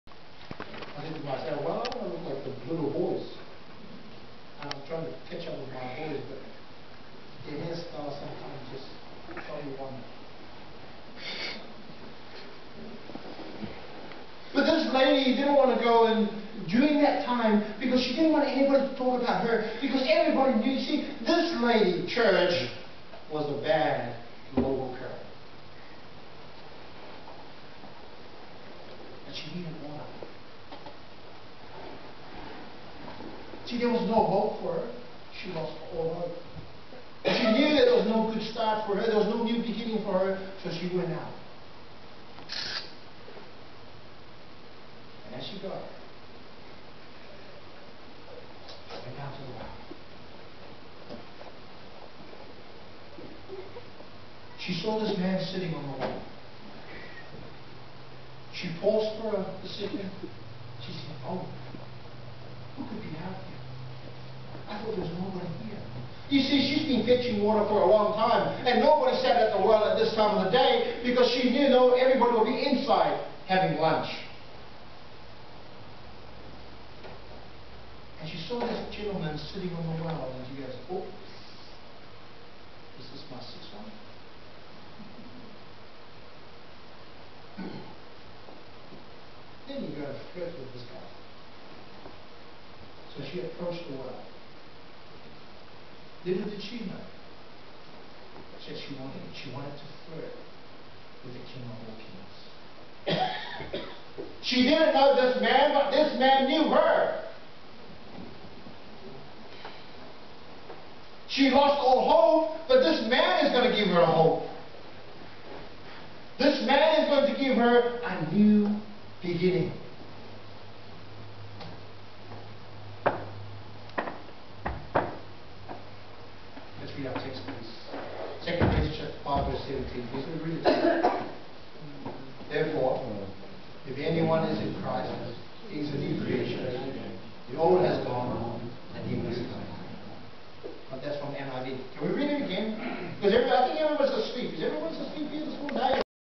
new beginning 2, Preaching at Dora Creek Church, Jan 10, 2009
John 4, the story of the Samaritan Woman and how she met Jesus at the well.  It was there she found forgiveness and a New Beginning in her life that will transform a whole village.